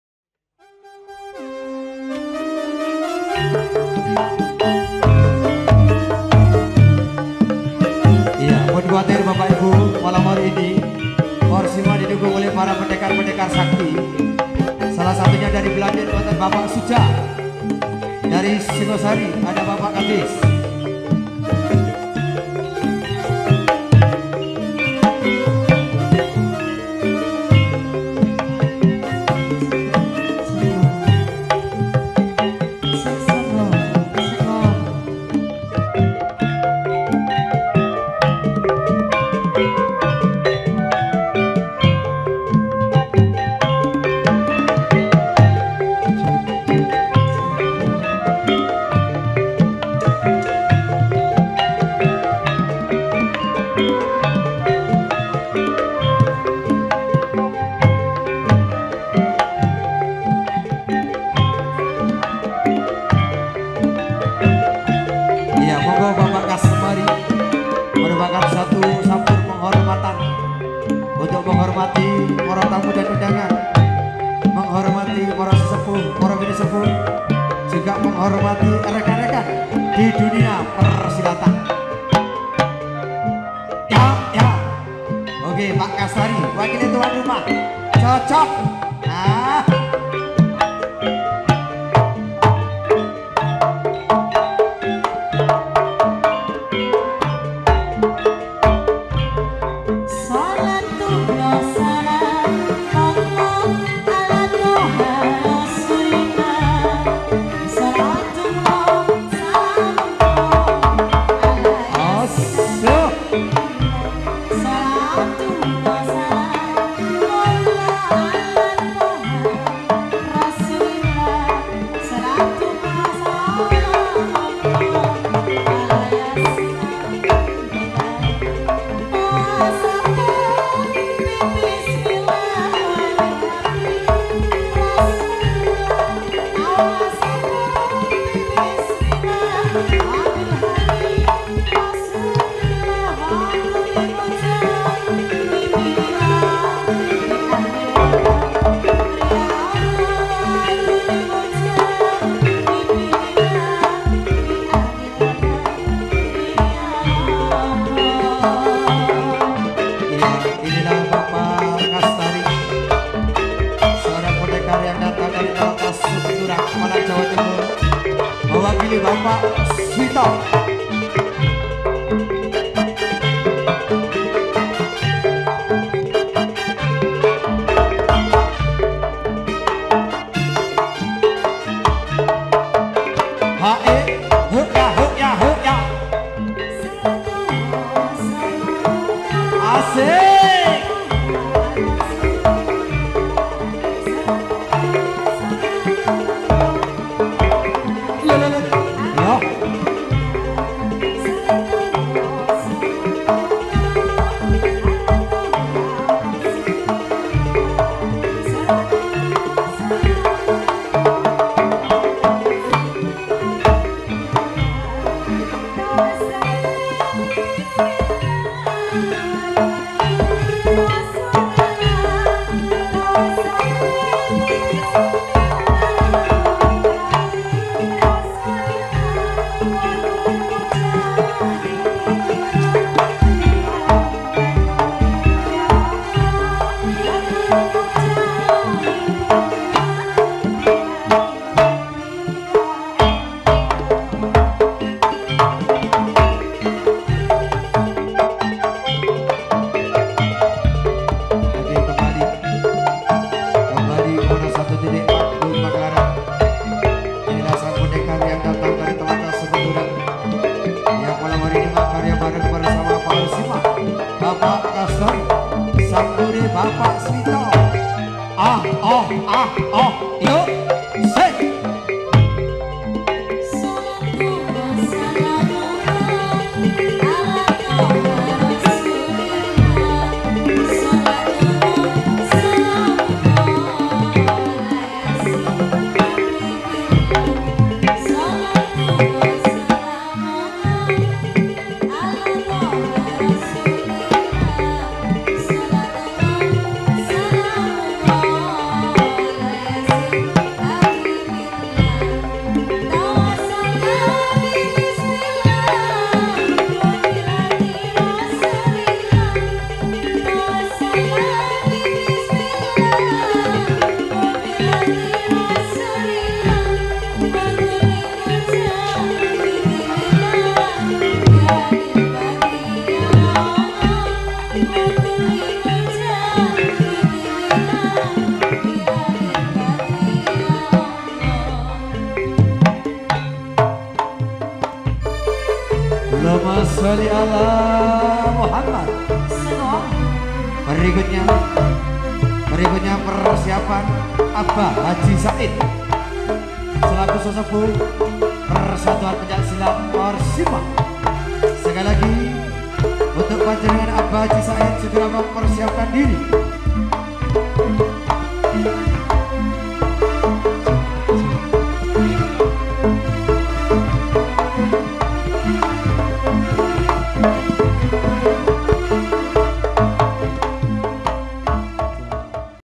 PENCAK SILAT TERBARU || PAORSIMA || KEMBANGAN BPK KASTARI
Atraksi ini adalah salah satu atraksi dalam pencak silat, bersama persatuan pencak silat paorsima bocek karangploso malang, para pesilat ini dengan sangat bagus membawakanya.